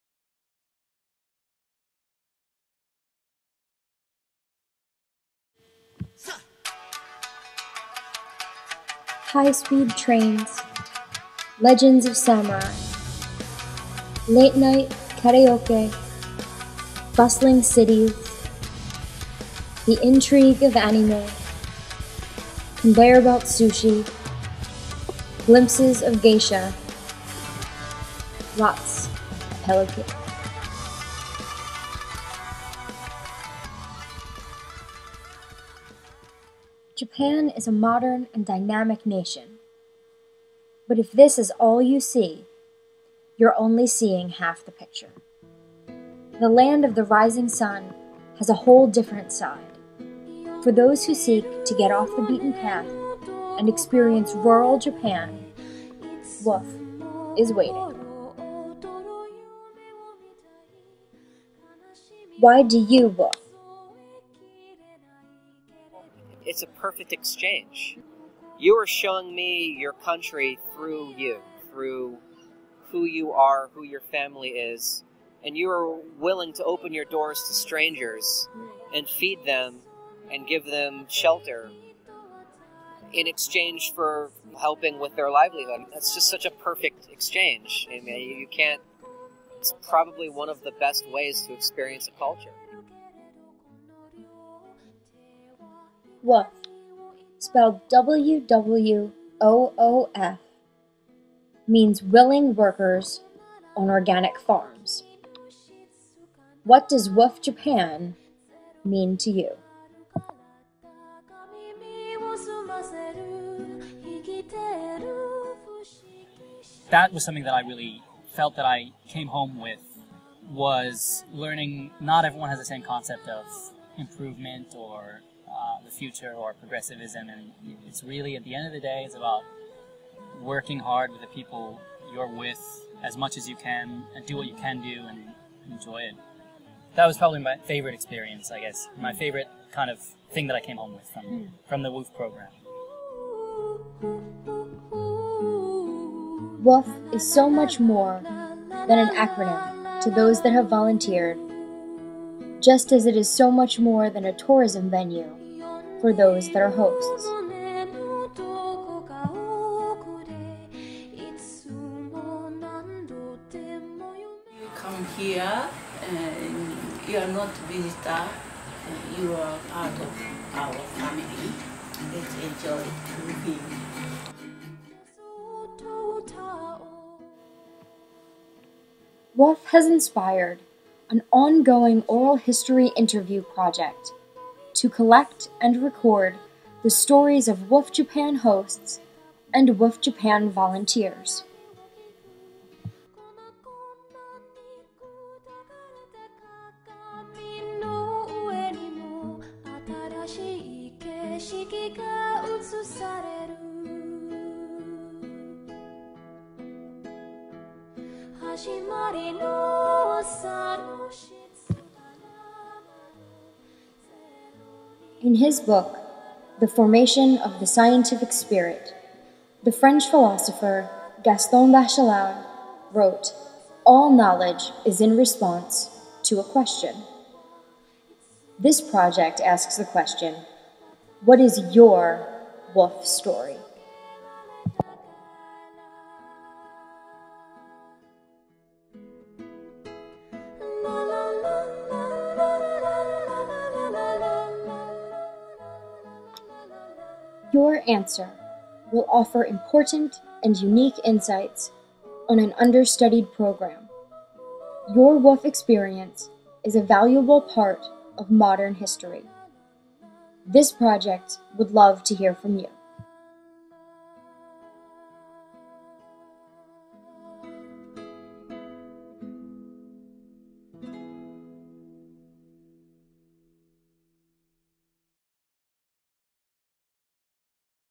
Oral History and WWOOF Japan
A description of my dissertation project with a request to interview WWOOF Japan participants